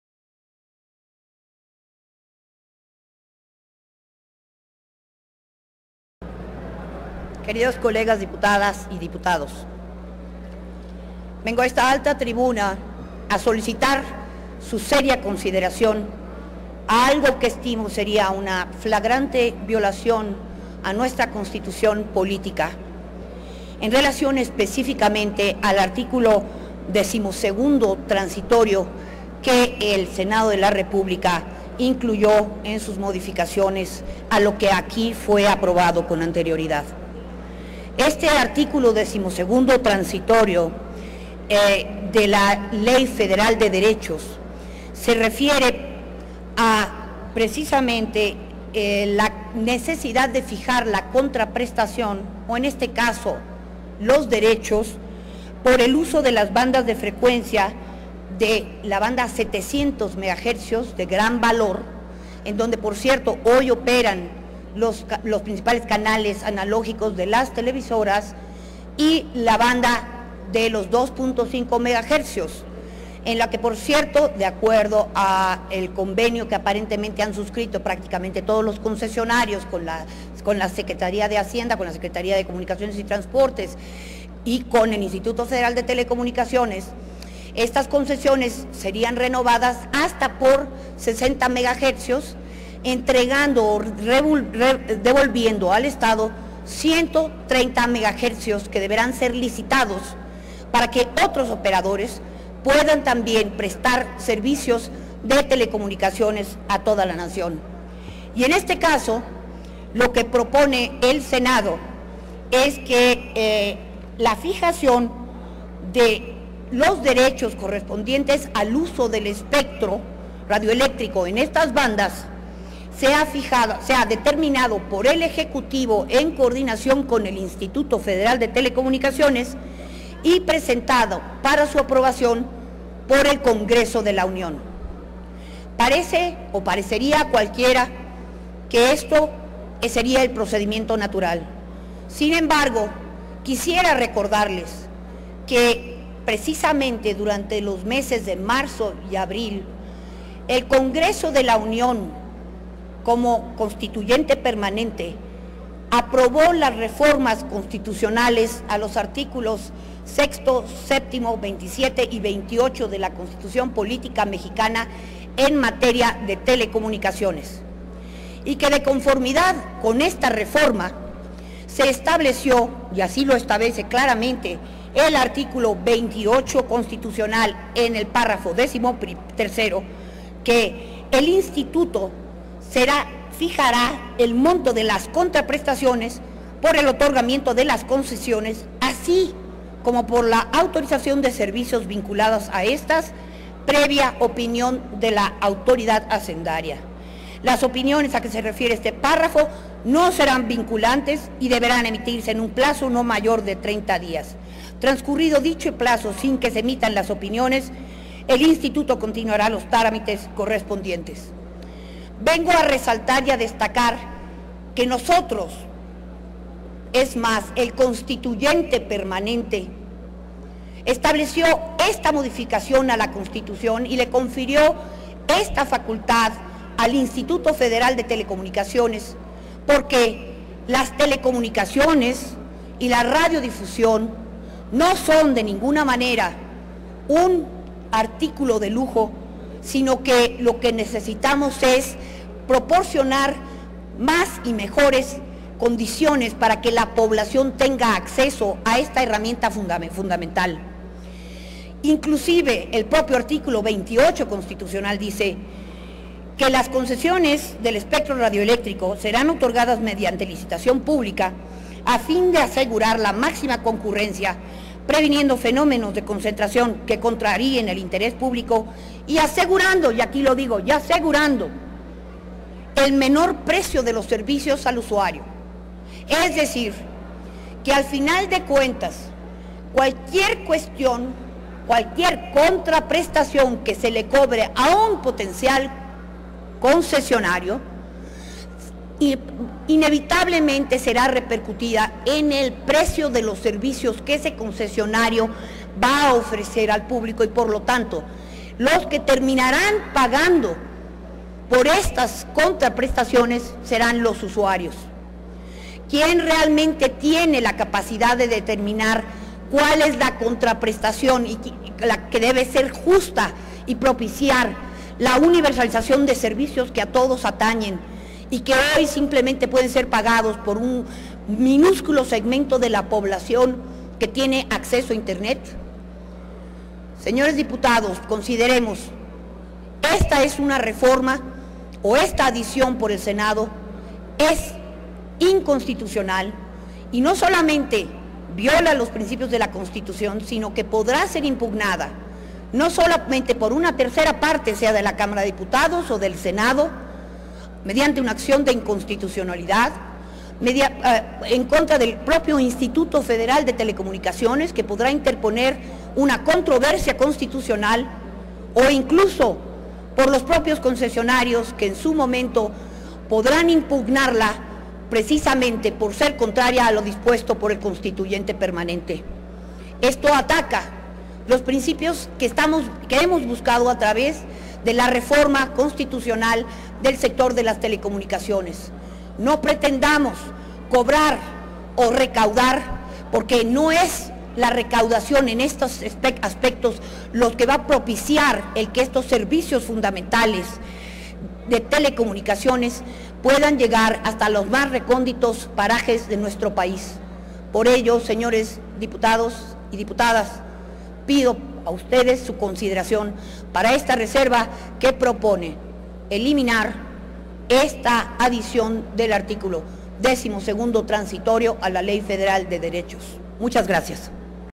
Dip. @PuriCarpinteyro reserva a Ley del IVA, IEPS, Federal de Derechos, ISR, IETU e IDE
La Dip. Purificación Carpinteyro Calderón solicita suprimir el artículo decimosegundo de la Ley de Derechos que faculta al Congreso de la Unión para establecer la contraprestación por el uso de la banda de frecuencia abierta. Recuerda que dicha facultad fue conferida al IFETEL y destaca lo dispuesto en la CPEUM en materia de telecomunicaciones. Asimismo, refiere lo dispuesto en el artículo 28 de la CPEUM y advierte que de aprobarse la modificación, ésta podrá ser impugnada por ser contraria a la disposición constitucional.